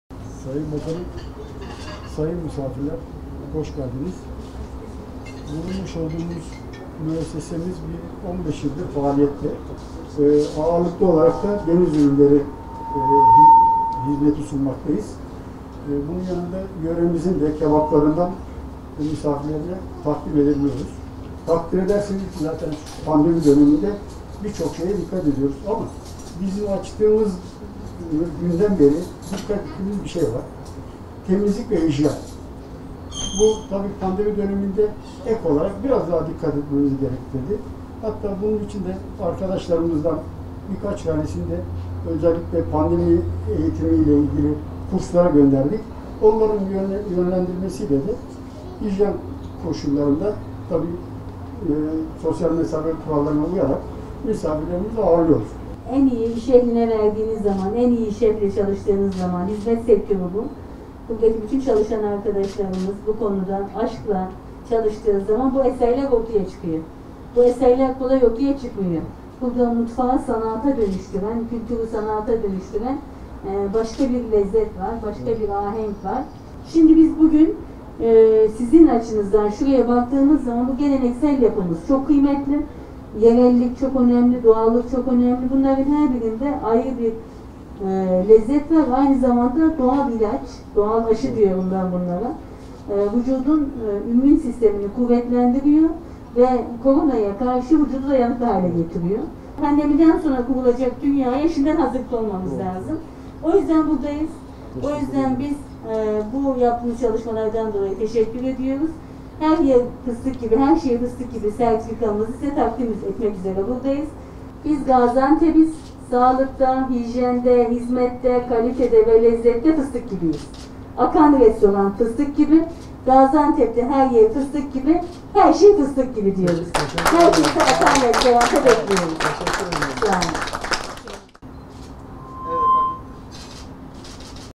AKAN RESTORAN DA FISTIK GİBİ OLDU
Başkanımız Fatma Şahin AğzımızınTadıGeldi kampanyasına katılıp hijyen eğitimlerini tamamlayan işletmelerin FıstıkGibi sertifikalarını takdim etti.

Akan Restoran da FıstıkGibi oldu.